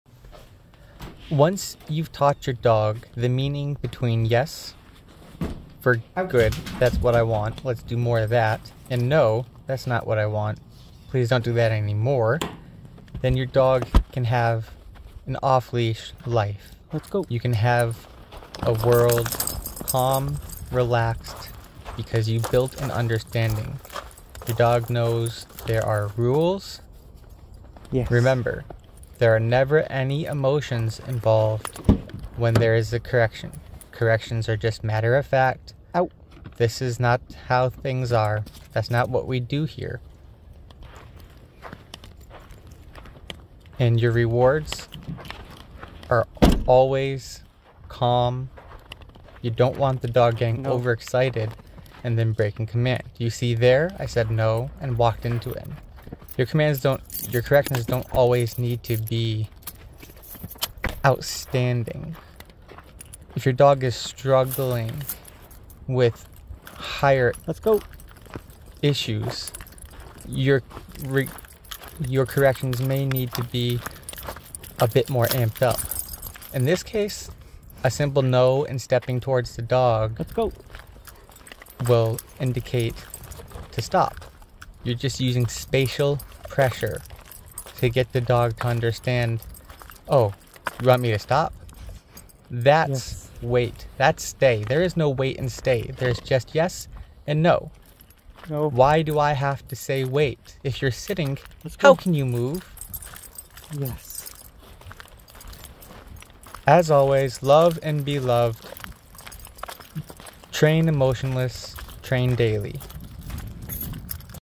There is now wait. there is no stay. there is only yes and no.
There is no wait. There is no stay. There is only yes and no. Keep your training very binary. Keep your corrections emotionless. Be careful with your rewards. An amped up reward can easily cause your dog to break command. Once you and your dog have a great understanding of yes, and no, you'll see a dramatic shift in your relationship and how your dog interacts with you and the world around it.